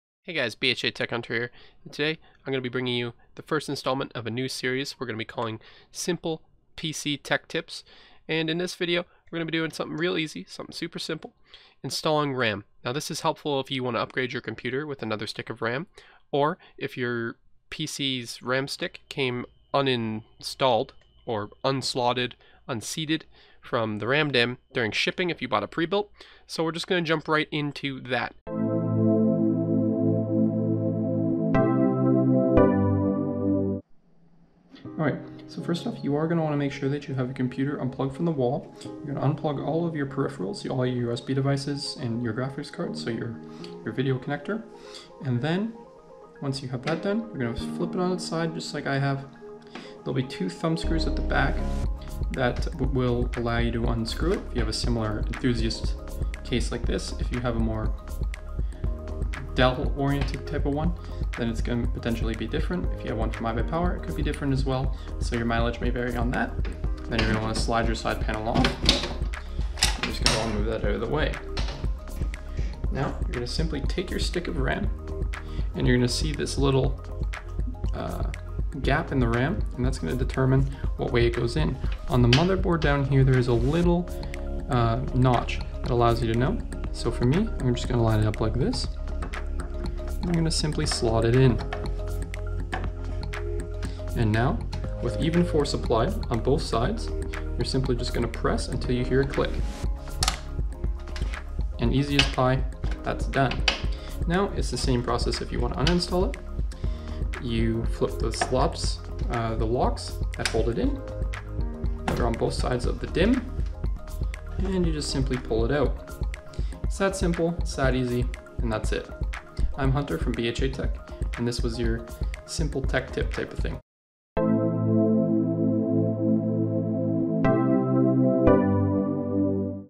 How To Install Ram - Simple PC Tech Tips
Today we are going to be showing you how to install ram, it's simple and easy so let's do it.